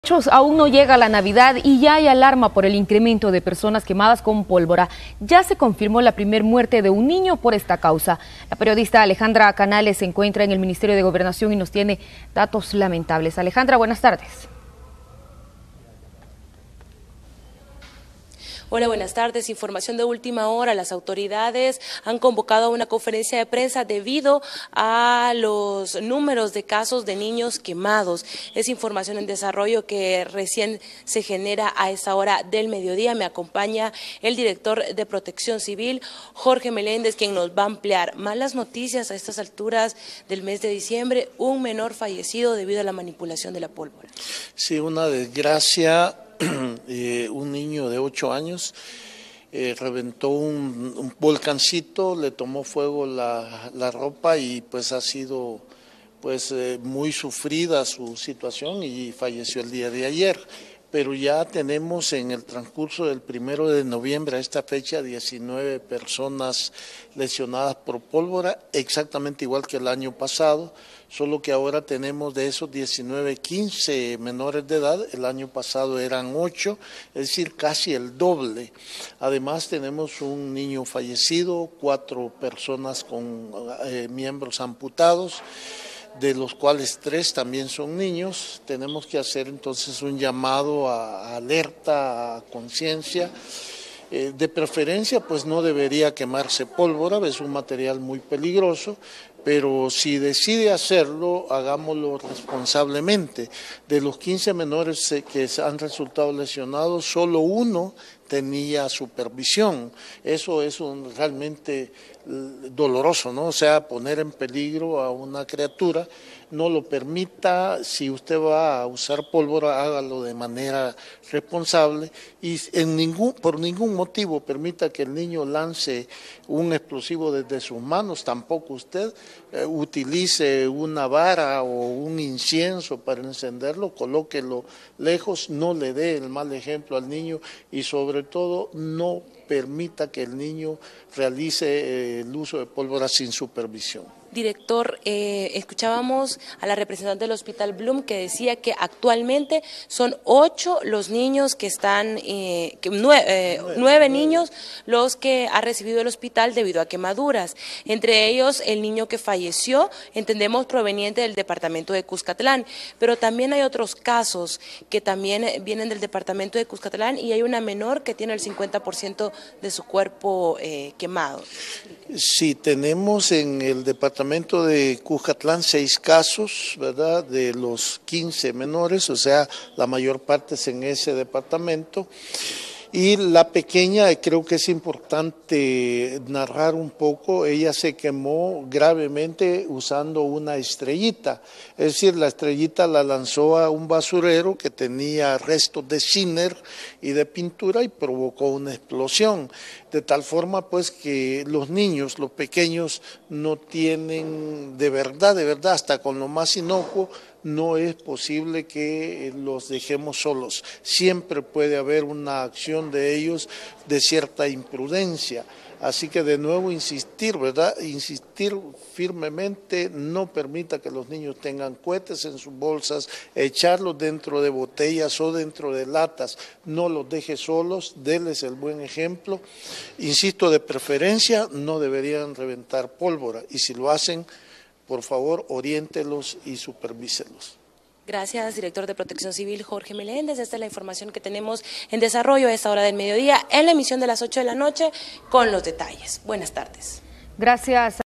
Se confirmó la primera muerte de un niño por pólvora en los festejos navideños
19 personas han resultado quemadas tras manipular pólvora en lo que van las fiestas de navidad.